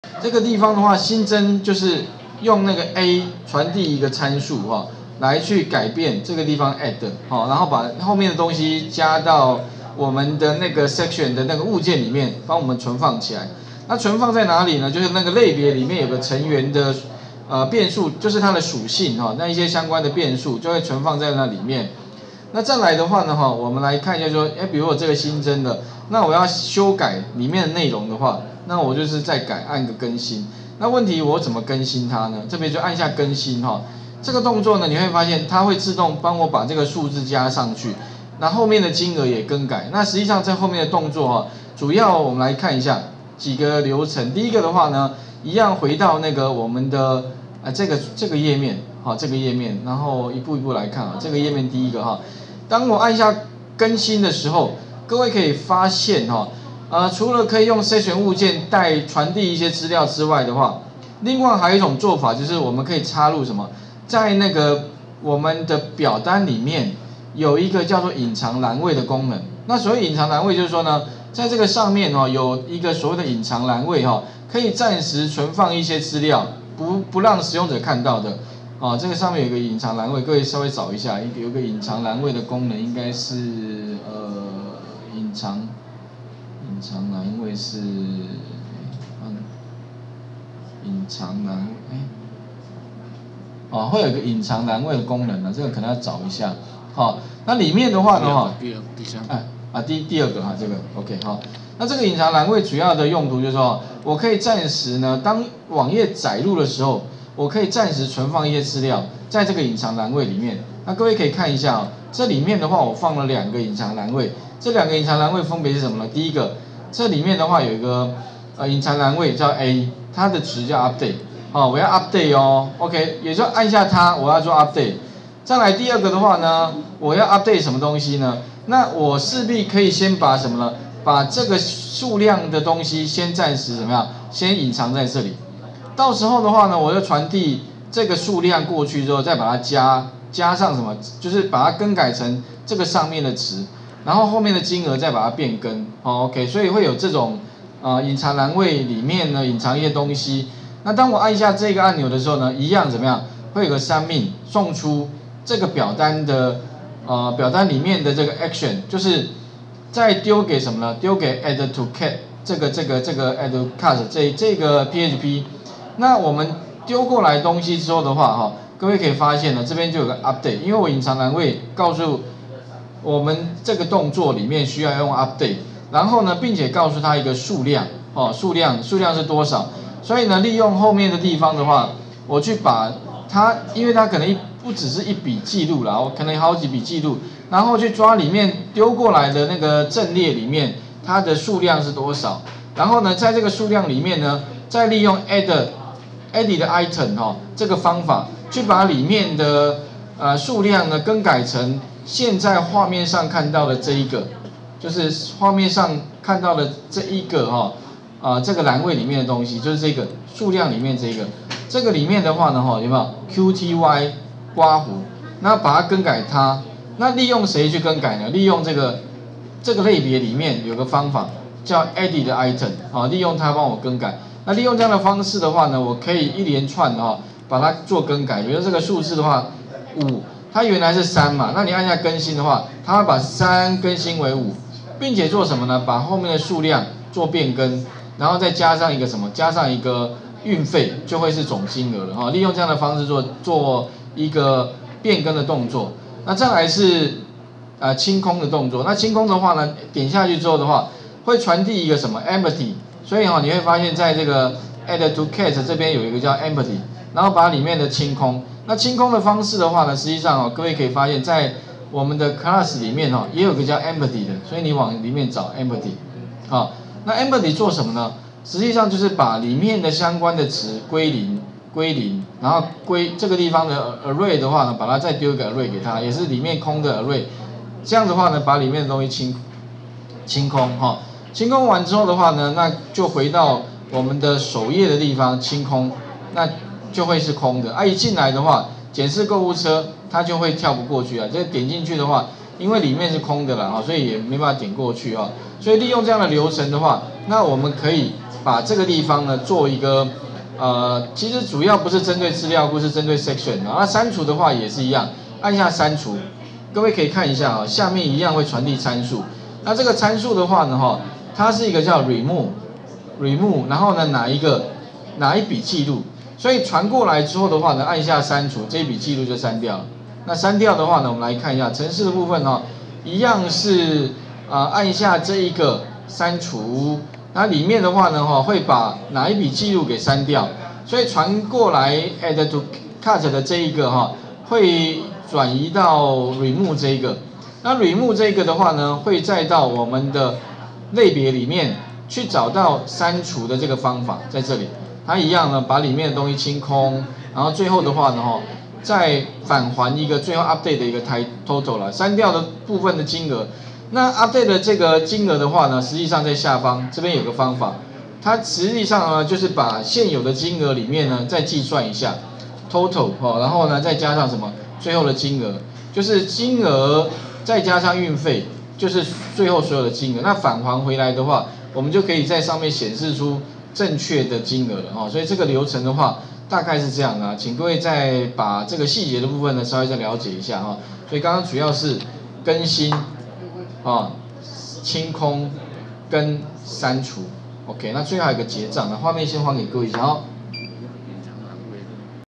05 如何更新或刪除與清空購物車
從PHP雲端資料庫到APP開發之11(PHP證照第2類_MYSQL)

上課內容：
01_如何在MYSQL建立會員系統資料庫
02_如何列出查詢系統中的會員資料
03_如何新增會員資料到資料庫中
04_如何修改與刪除會員資料
05_下星期APP課程預告